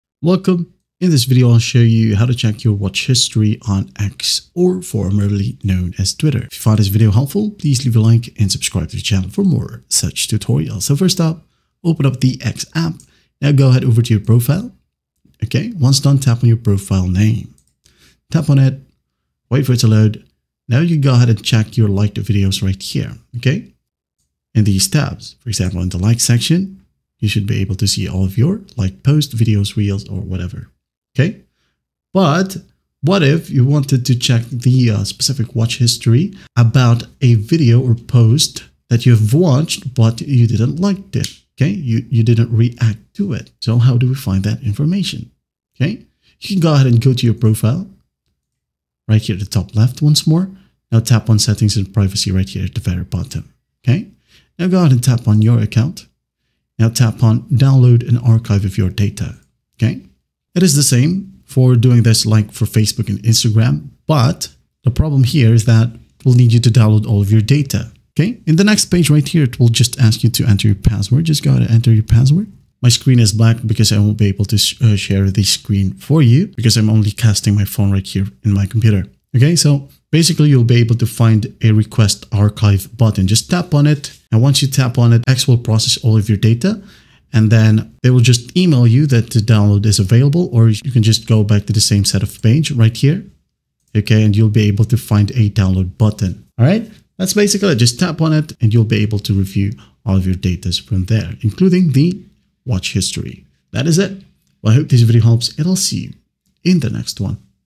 How To Check Watch History On X / Twitter (Quick Guide 2026)
How To Check Watch History On X / Twitter

In this video, I will show you how to check your watch history on X (formerly Twitter). If you’ve been scrolling through videos and want to find something you watched earlier, this guide is for you. I’ll walk you through the steps to access your history using X settings, explain how recommendations are influenced by your viewing activity, and show you alternative ways to track recently watched videos. By the end, you’ll know exactly how to review your watch history and manage your activity on X / Twitter.

Chapters:
00:00 Introduction
00:10 How To Check Watch History On X / Twitter